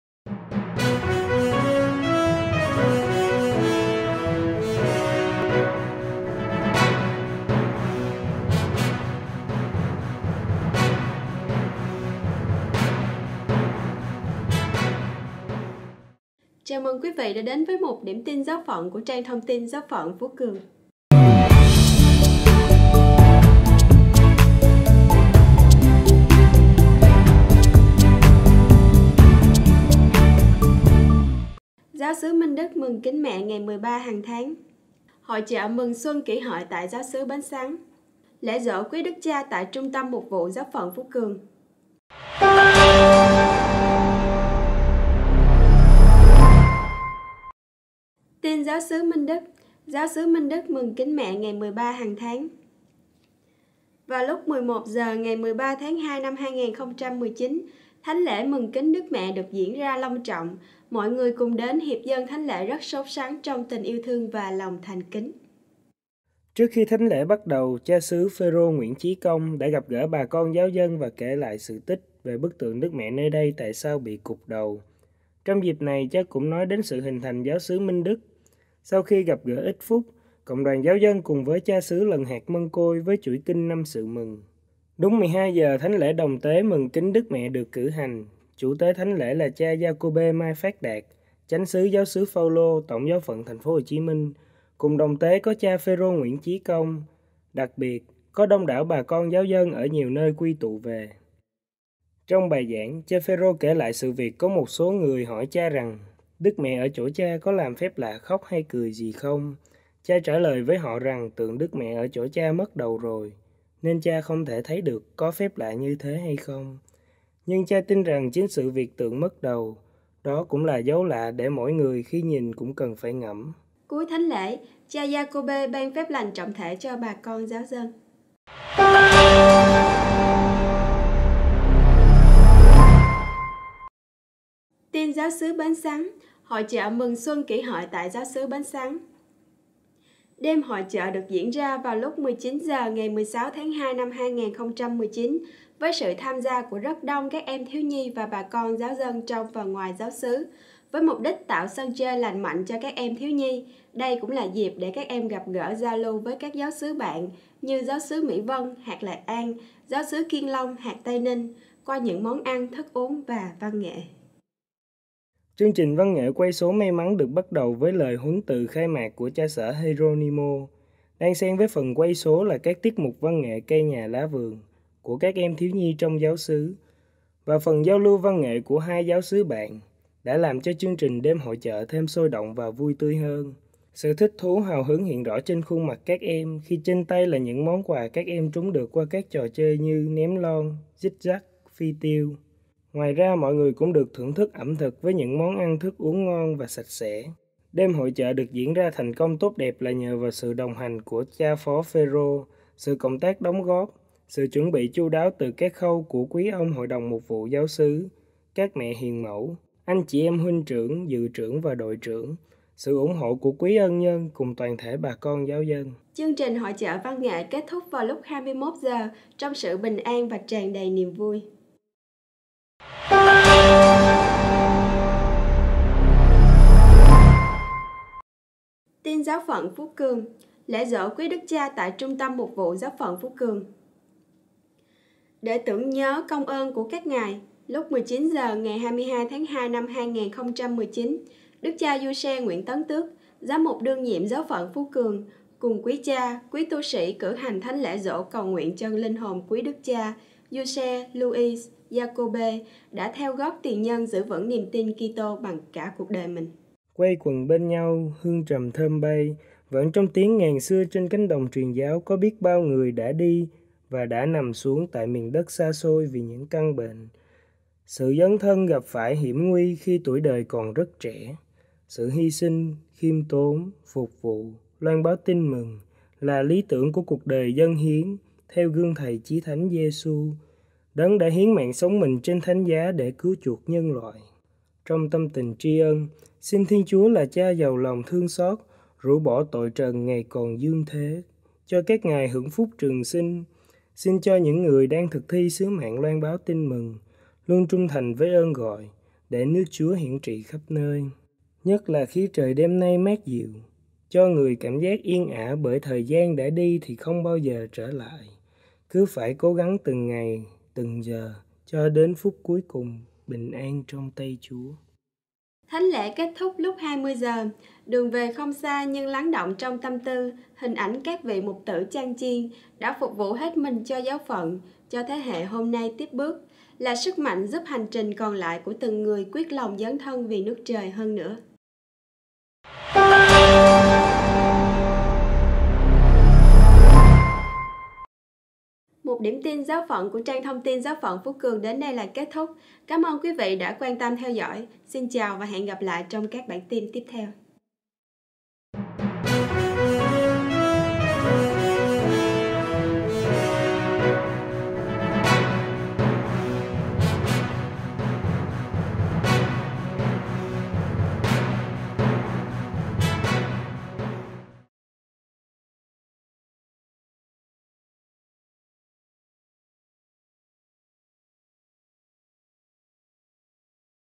ĐIỂM TIN TRONG TUẦN GIÁO PHẬN PHÚ CƯỜNG 17.02 ĐẾN 24.02.2019
1. TIN GIÁO XỨ MINH ĐỨC: Giáo Xứ Minh Đức Mừng Kính Mẹ Ngày 13 Hàng Tháng.
2. TIN GIÁO XỨ BẾN SẮN: Hội Chợ Mừng Xuân Kỷ Hợi Tại Giáo Xứ Bến Sắn.
3. TIN GIÁO PHẬN PHÚ CƯỜNG: Lễ Giỗ Quý Đức Cha Tại Trung Tâm Mục Vụ Giáo Phận Phú Cường.